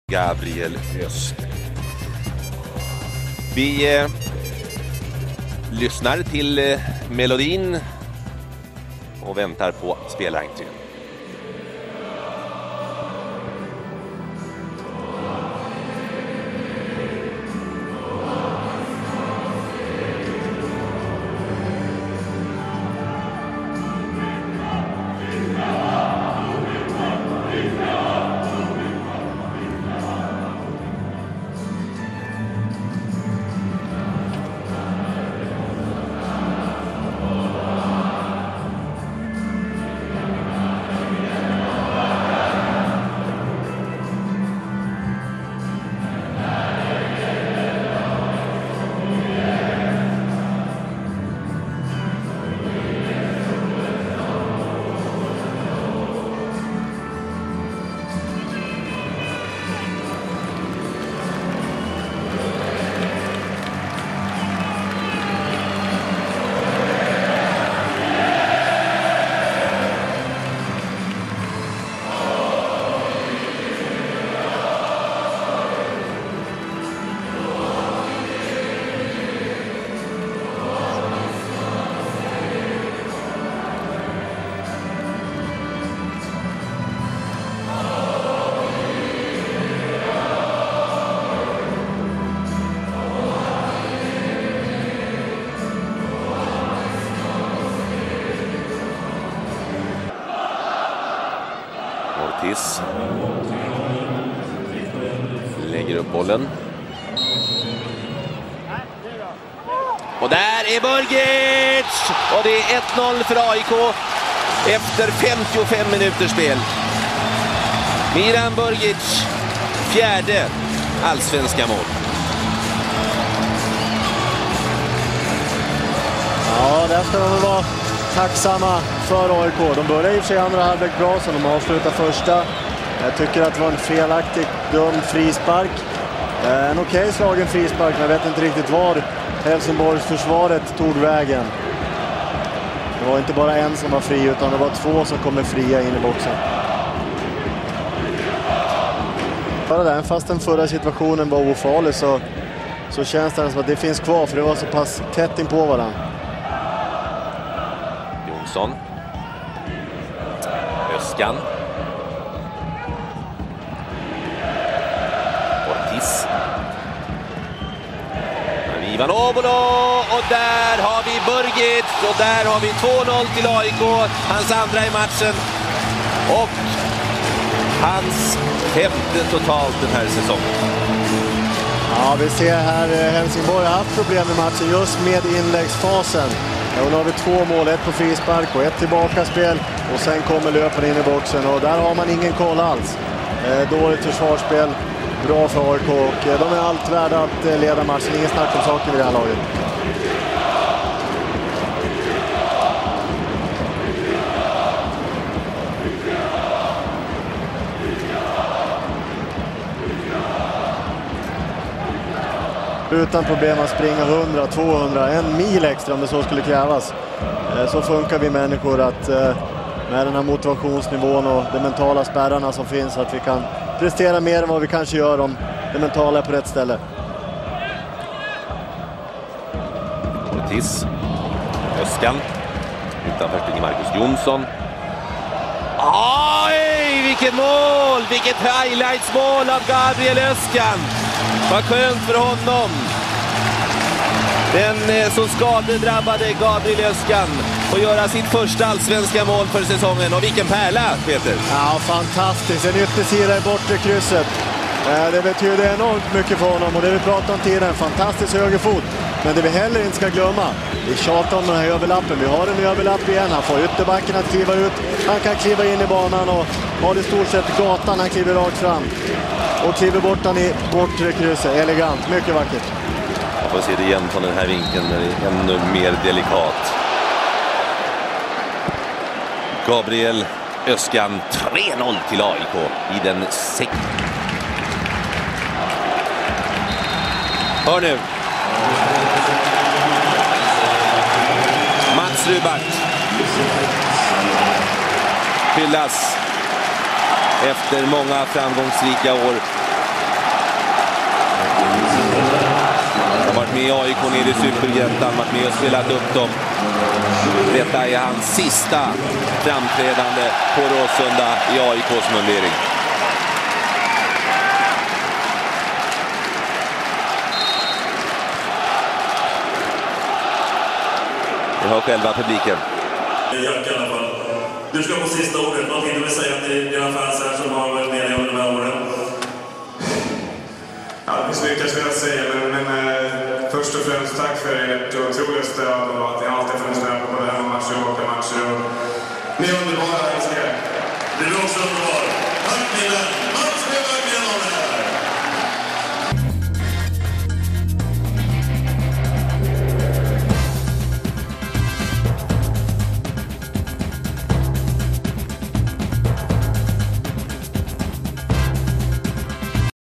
AIK - Helsingborgs IF (2008) (Målen och lite Rubarth)
AIK:s mål och även lite Mats Mats Rubarth!